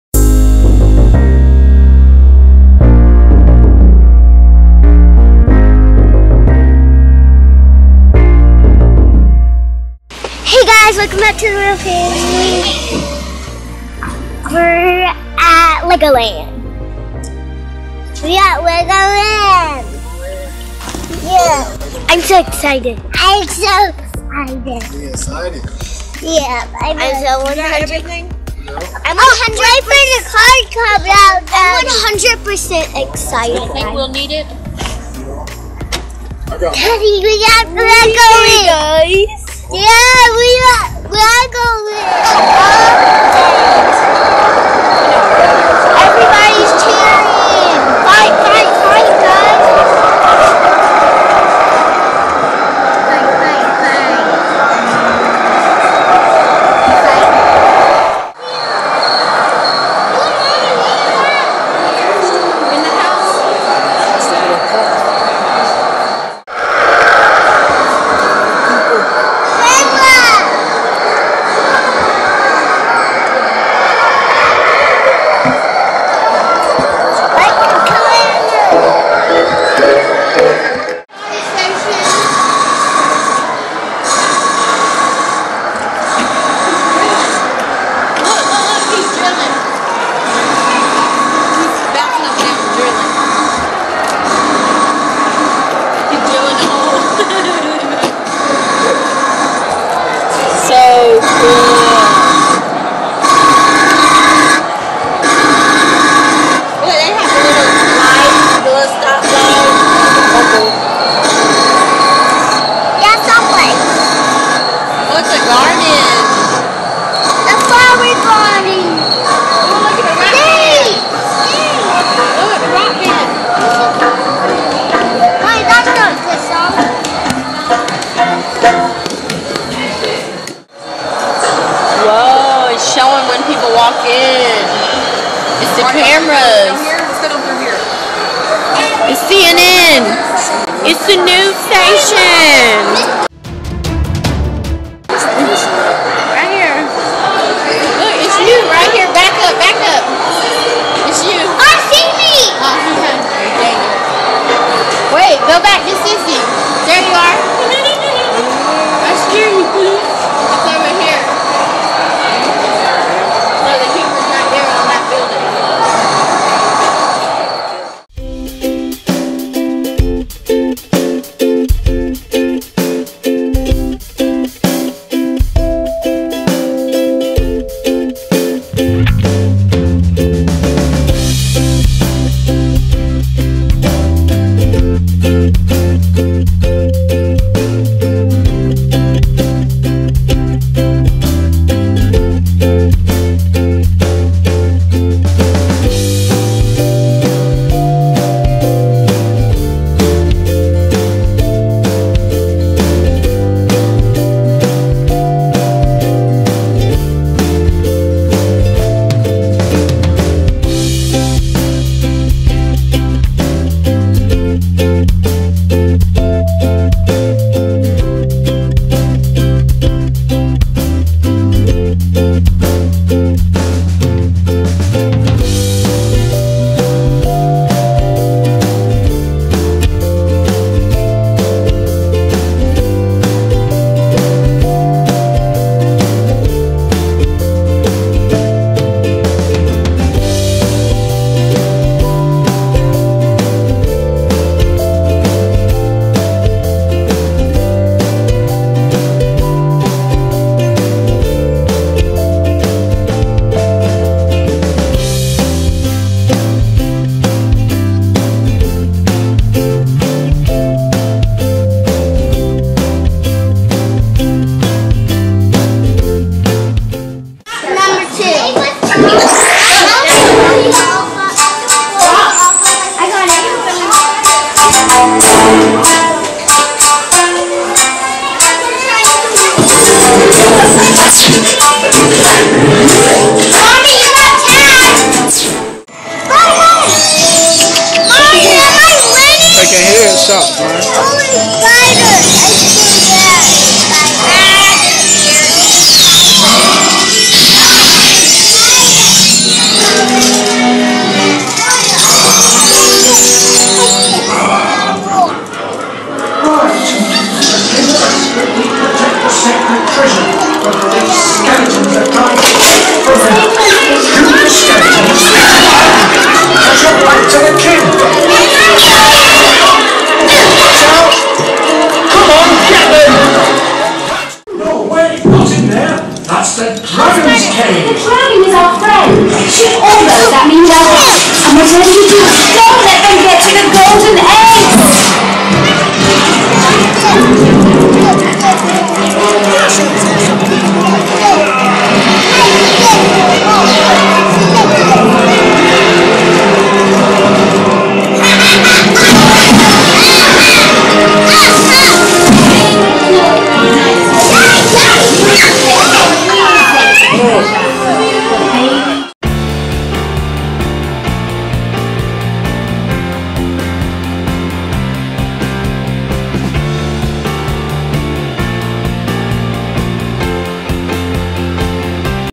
Lego Land Adventure
Join us with fun at Lego Land.
We find diffrent legos to build.
Take picture with lego Characters.